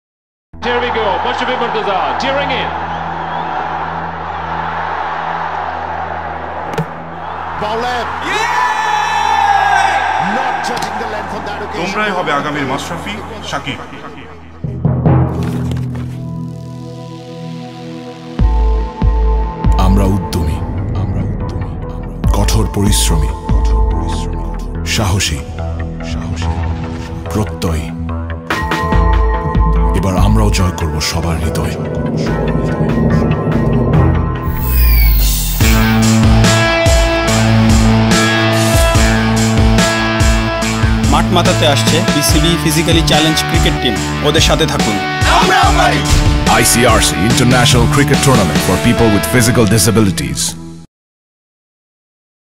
Bangladesh Cricket tournament for people with physical disabilities
This video showcases the willpower of Bangladesh's Physically Challenged Cricket Team, and their brave and indomitable players.
The event, organised in close partnership with the Ministry of Youth and Sports, Bangladesh Cricket Board and Bangladesh Krira Shikkha Protishtan, is also supported by Mashrafe Bin Mortaza, captain of Bangladesh National Cricket Team.